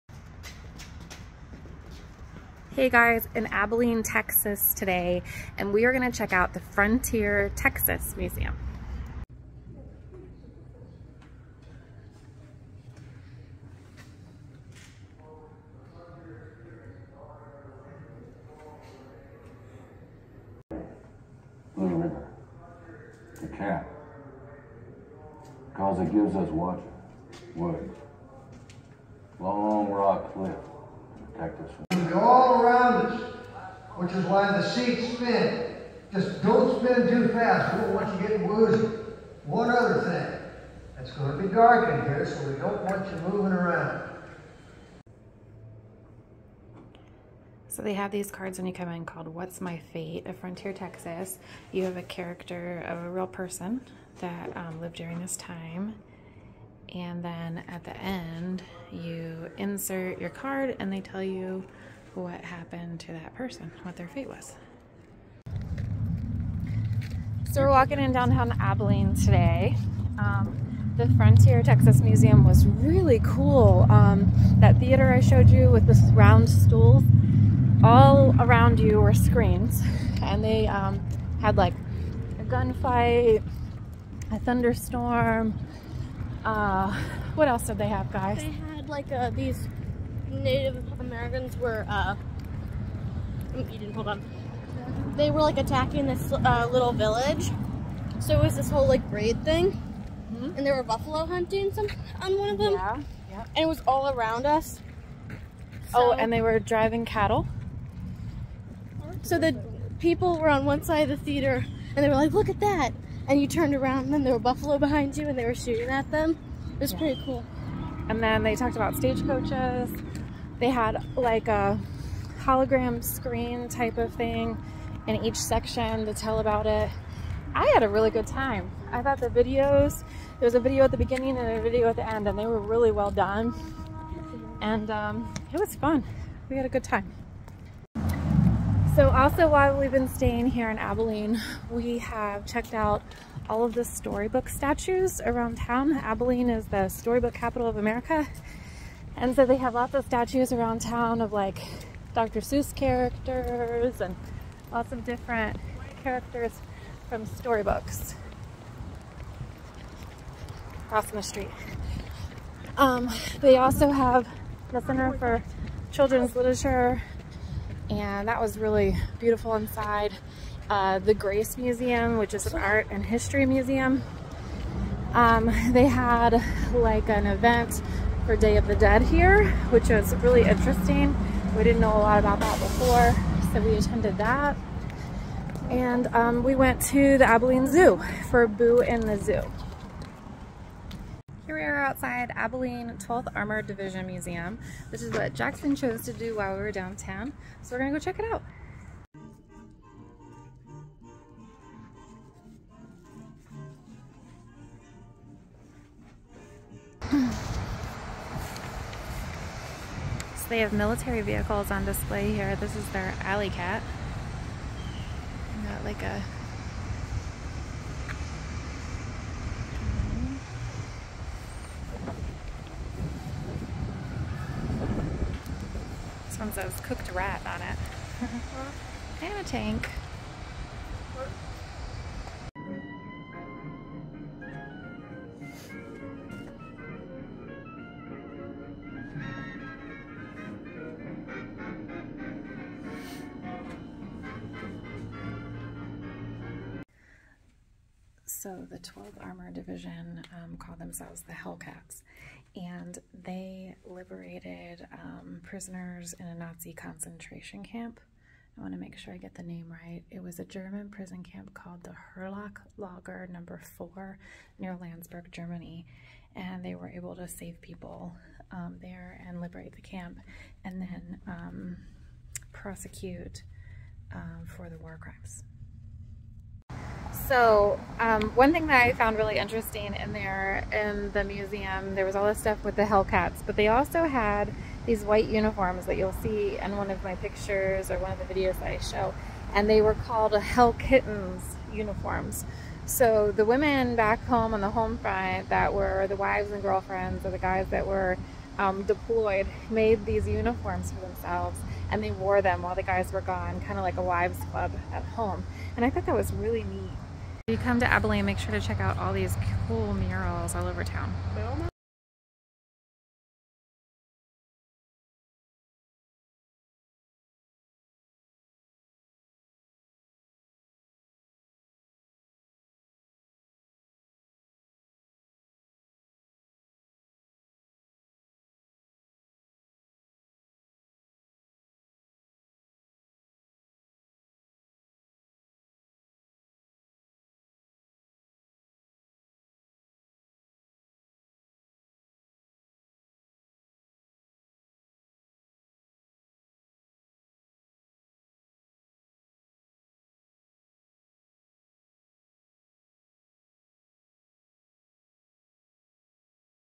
Abilene, Texas, a great place to visit.
Full-time RV family traveling America visits Abilene, TX.